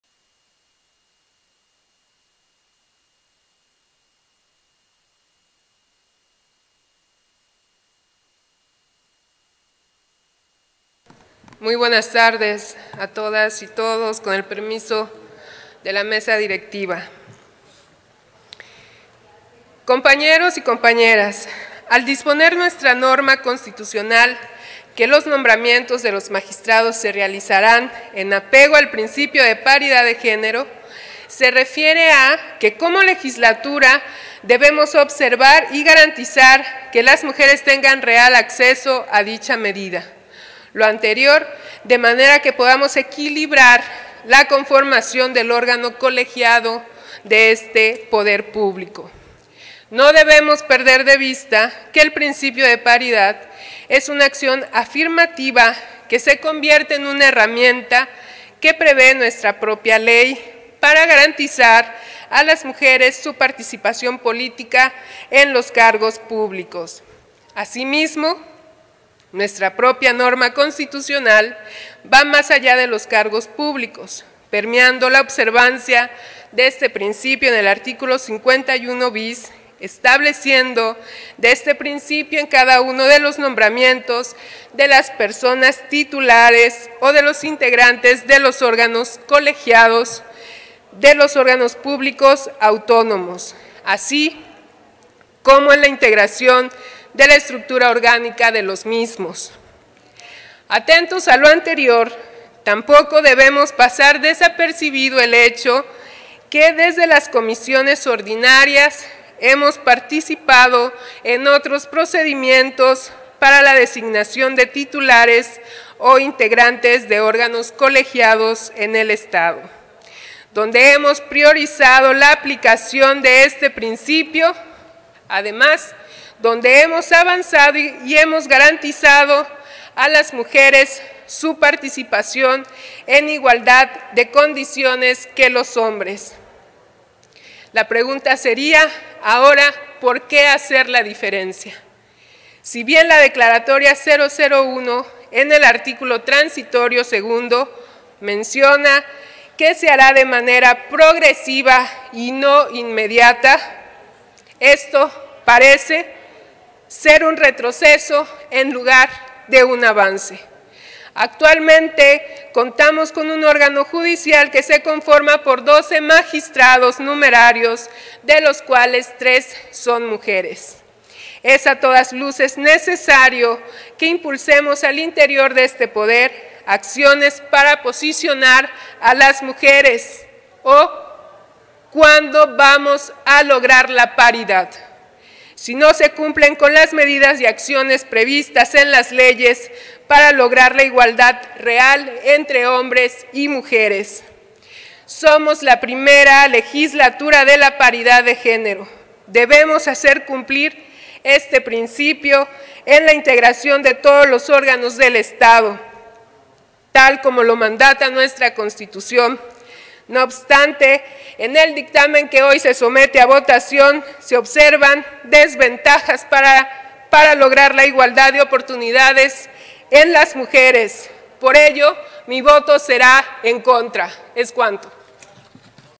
Intervención de la Dip. Linda Saray Cobos Castro. MORENA (29/Nov/2021)
Intervención de la Diputada Linda Saray Cobos Castro, Presidenta de la Comisión de Desarrollo Rural y Pesquero de la XVI Legislatura, en relación al Dictamen de la Comisión de Justicia de la H. XVI Legislatura del Estado Libre y Soberano de Quintana Roo, que contiene los nombres de la candidata y los candidatos de la terna a ocupar el cargo de Magistrado Numerario del Tribunal Superior de Justicia del Estado de Quintana Roo, que cumplen con los requisitos previstos en el artículo 101 de la Constitución Política del Estado Libre y Soberano de Quintana Roo.